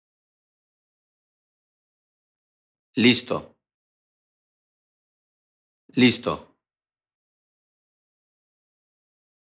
How to pronounce LISTO in European Spanish
This video show you how to pronounce LISTO in European Spanish. Speaker has an accent from Castellón (Valencia region, Eastern Spain). Collins Dictionary, the home of living language and pioneers of dictionary publishing